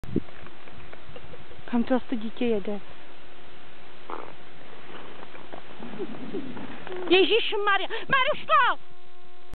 Lyžování
Jo... já vim, jedu strašně a jsem příšerně zakloněná. Ale carve se na bigfutách moc jezdit nedá.  Ty kratoučký lyžičky vůbec nezržel stopu... :-( . To jsou lyže na blbnutí, ale na sjezd ne...
Jinak videjko je točený na Tognole (Dolomity).